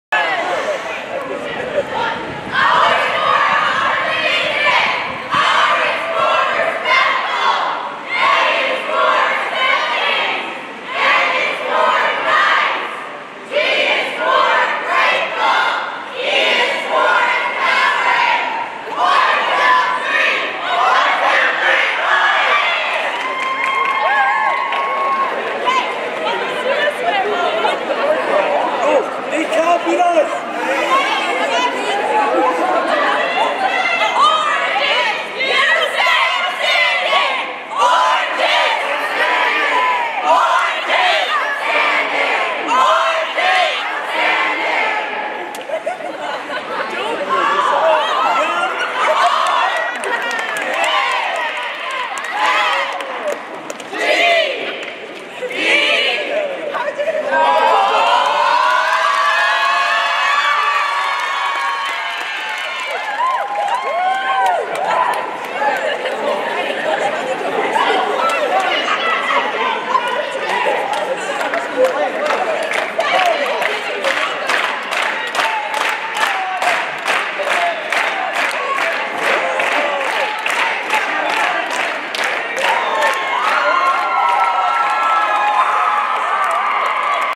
Orange Section Cheer 2024
At seminar, each color section is asked to work together to create their own section cheer. Here is the Orange Section Cheer!